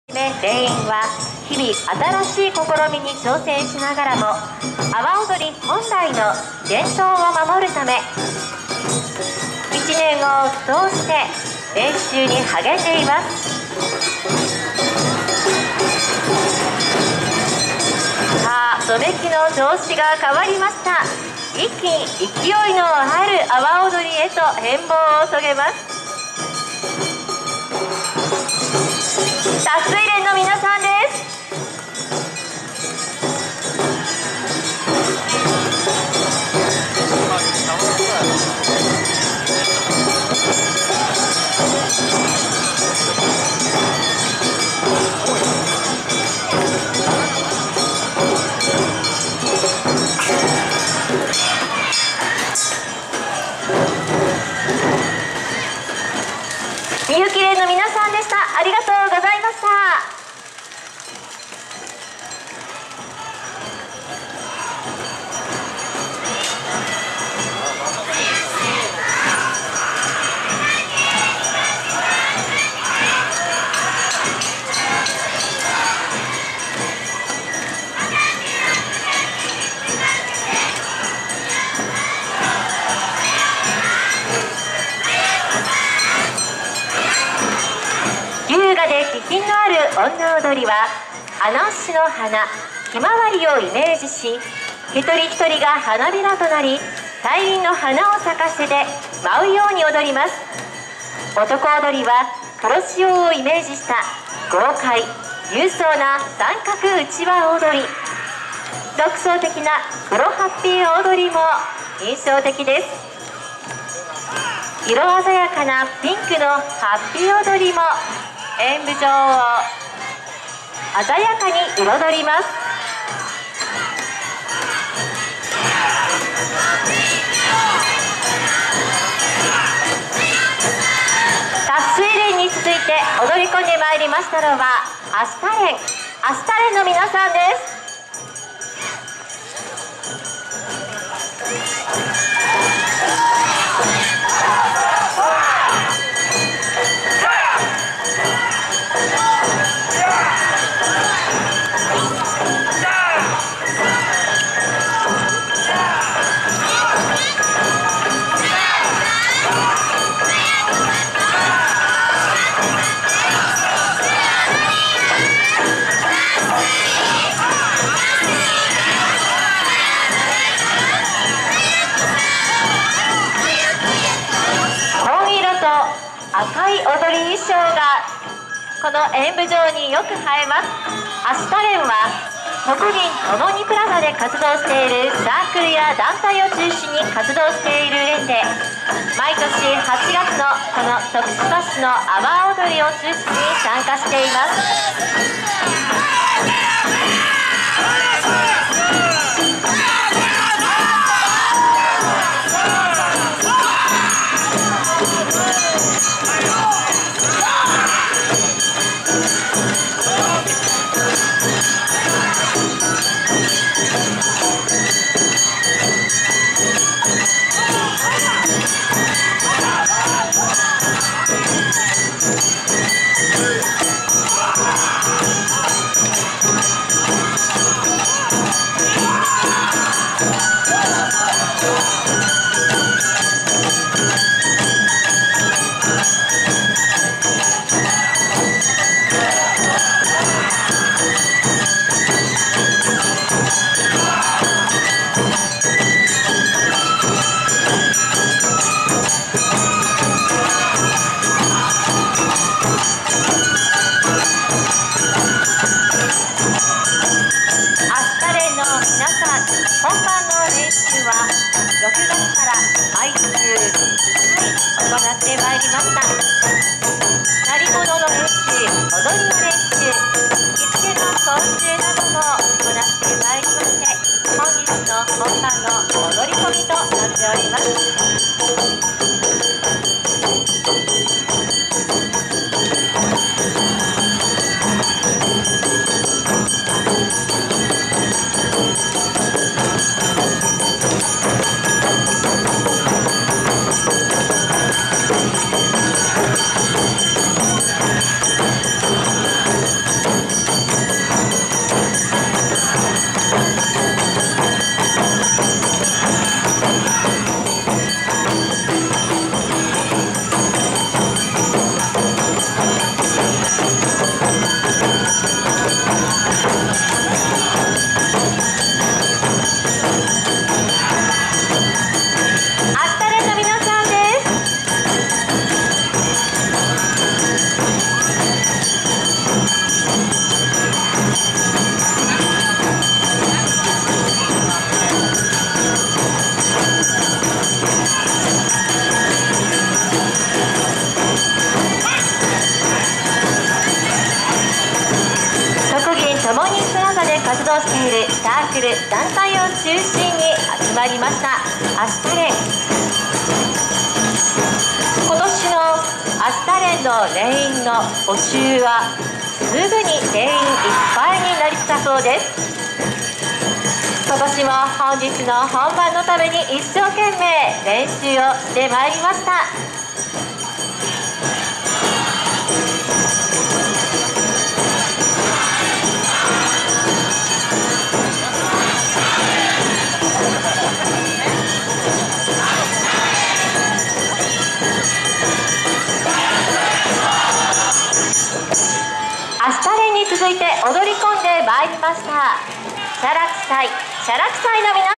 達粋連／両国本町演舞場／徳島市阿波おどり2016
2016/08/14
徳島市阿波おどり2016（三日目）
【会場】徳島市内各所の有料演舞場、無料演舞場、おどり広場ほか
【主催】徳島市観光協会、徳島新聞社
【主管】阿波踊り実行委員会
【後援】徳島県、徳島市、徳島商工会議所